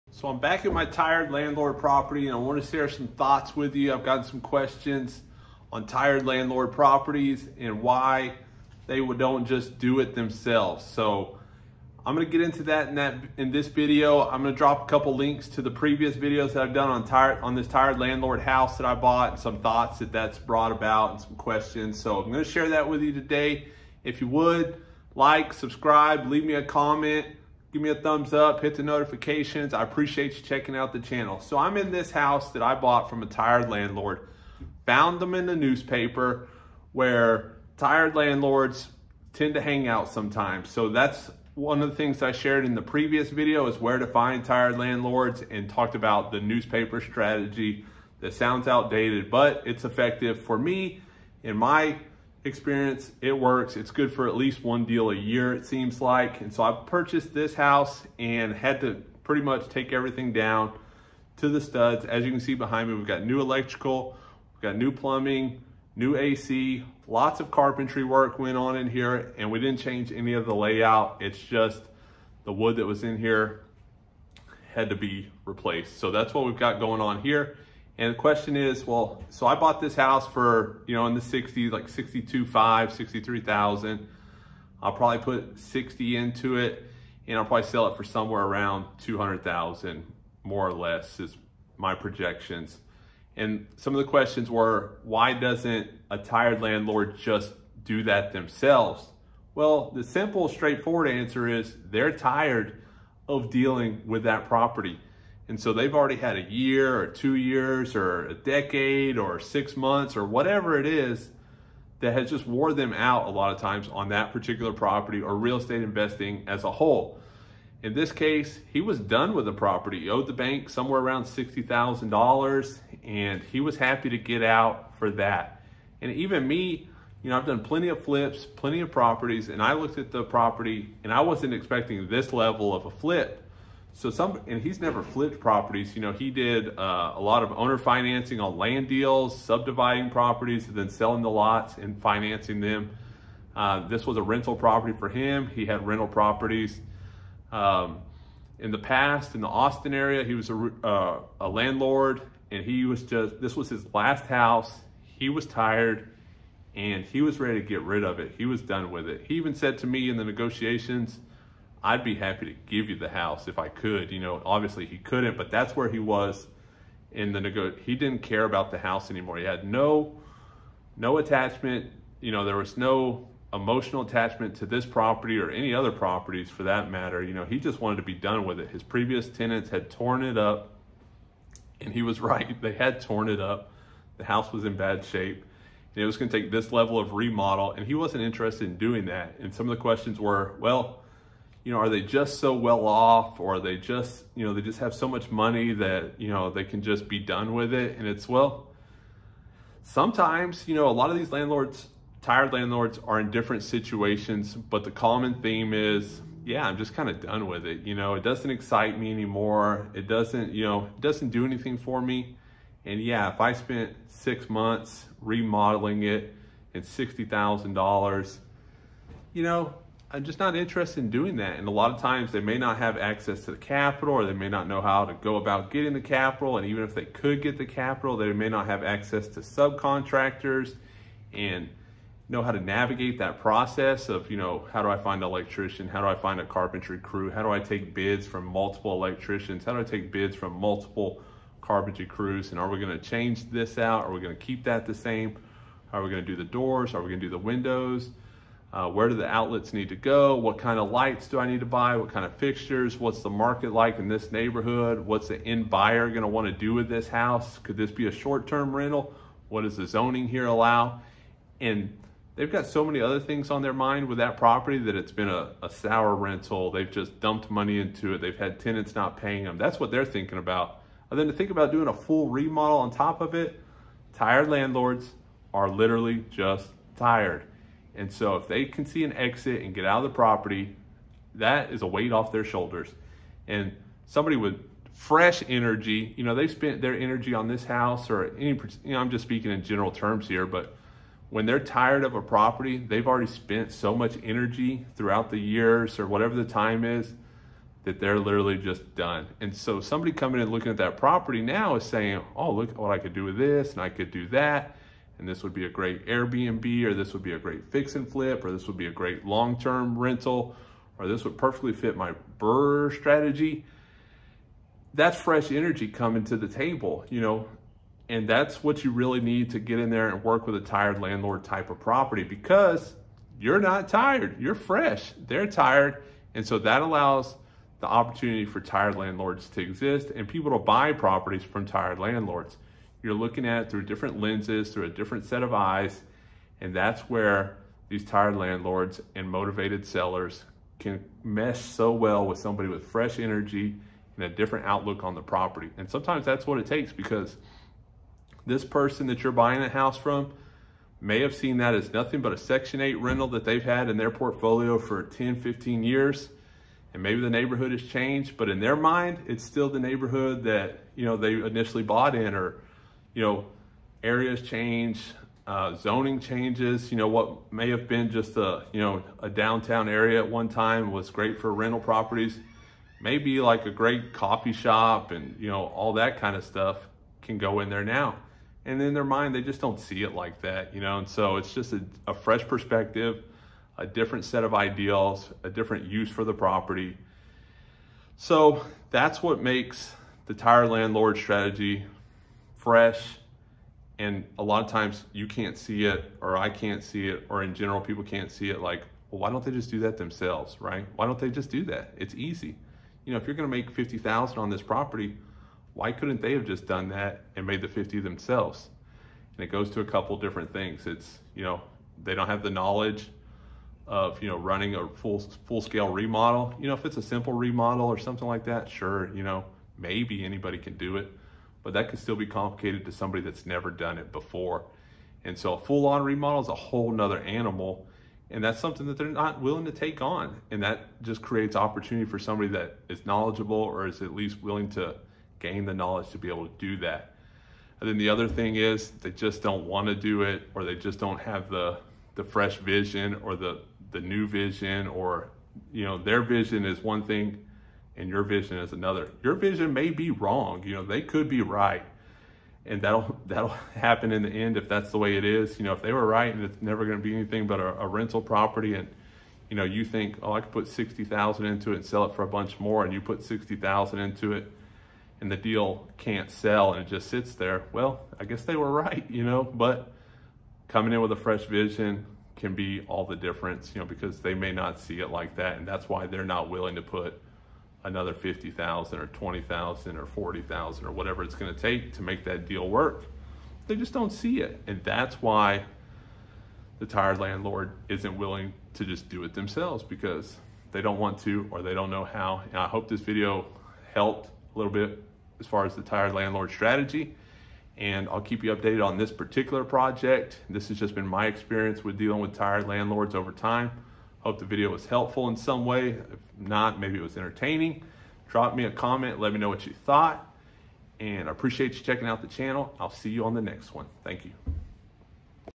Why dont tired landlords flip their own properties Motivated sellers are not motivated flippers
This is a question I have gotten many times before, why would someone who knows the house could be worth more money not just do a remodel and fix it themselves and get the "extra money," well as easy as a remodel looks on tv it is a daunting task more often than not and someone who is already tired of a property probably isn't looking forward to spending more time, energy, and money doing a remodel on a piece of real estate they have disconnected from.

thanks for checking out the channel and some of the videos.